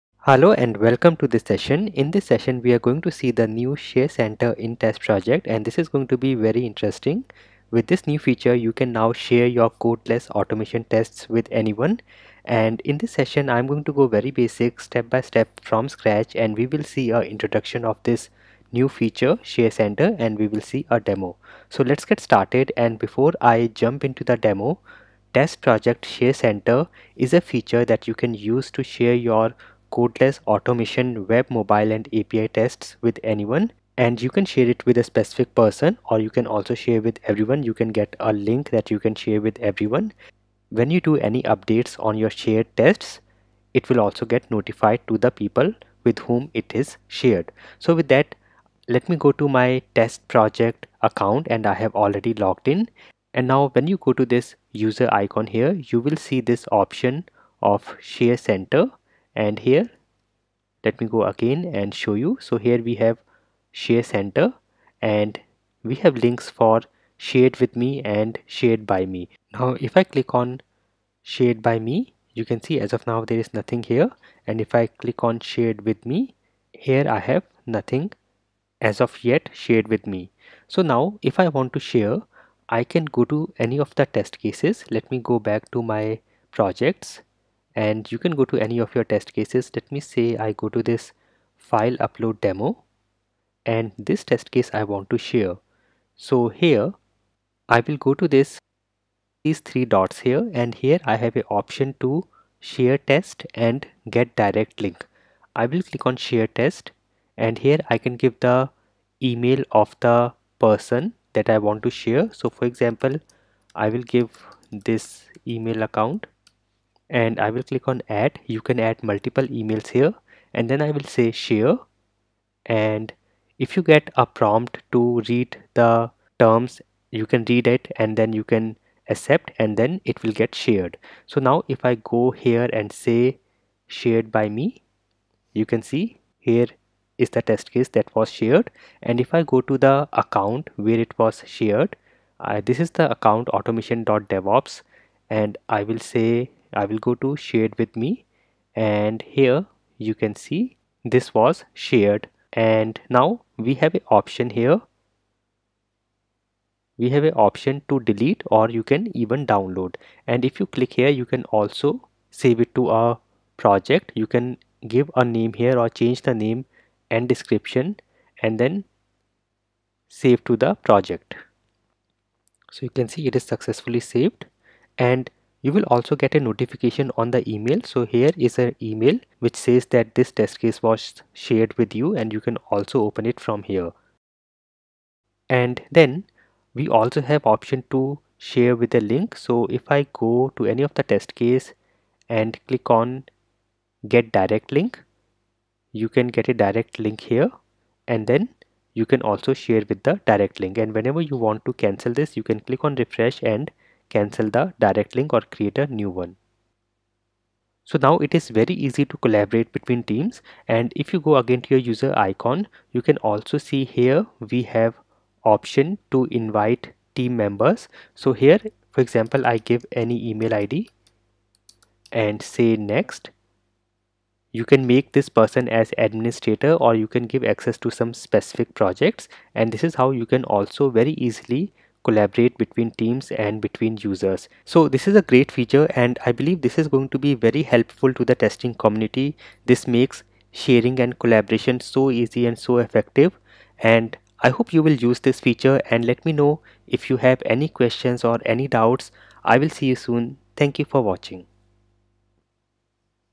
TestProject Tutorial 20 | Introducing Share Center
Introduction - TestProject Share Center
    Demo

Share web, mobile, api tests with anyone with a single click
Can share with specific people or everyone
Any updates on the shared test(s) are notified

Raghav